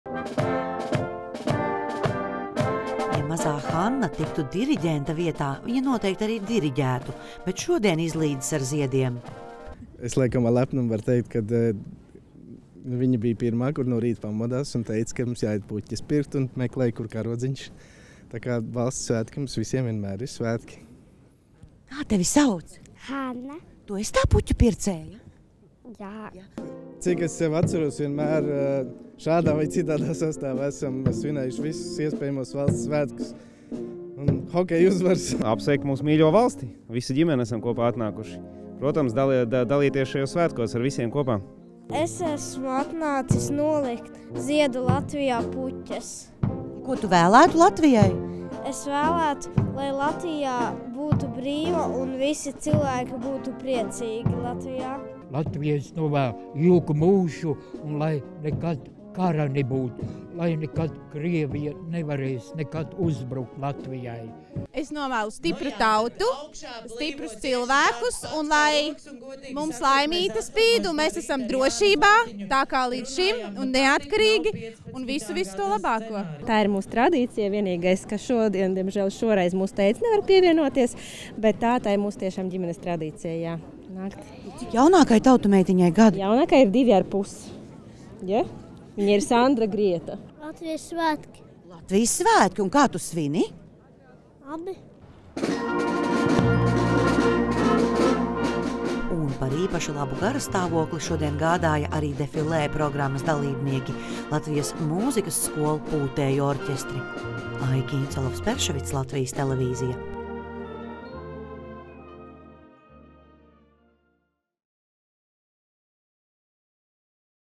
Latvijas ģimenes: Svinēt 4. maiju ir tradīcija!
Tāpat kā iemācām bērniem teikt lūdzu un paldies dzimtajā valodā, tāpat jāmāca 4. maijā sveikt Latviju tās svētkos. To atzīst ģimenes, kuras šodien sastapām Brīvības laukumā, apmeklējot Ziedu Latviju un ieliekot savu puķīti valsts kontūrā. Tikai tā visi kopā varam panākt, ka visa Latvija ir ziedoša. Tiešā un pārnestā nozīmē.